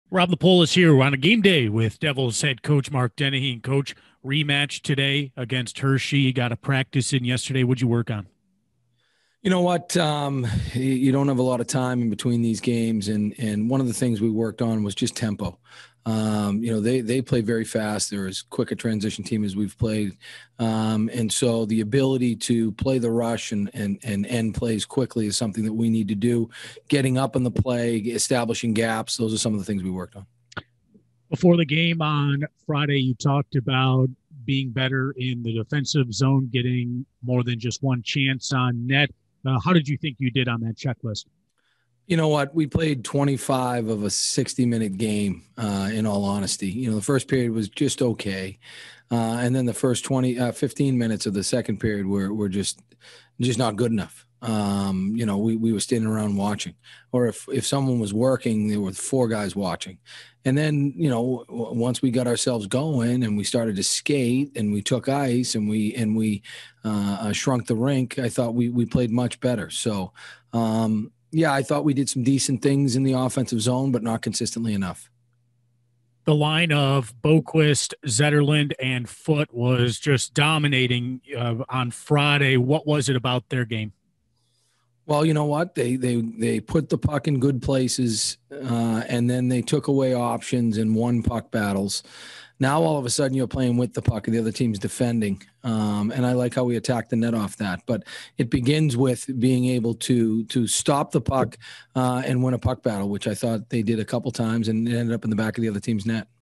Mark Dennehy Pregame - 3/21/21
We're all set for today's rematch in Hershey!

♦️ Tempo in practice yesterday
♦️ Skating more & taking ice
♦️ Boqvist/Zetterlund/Foote line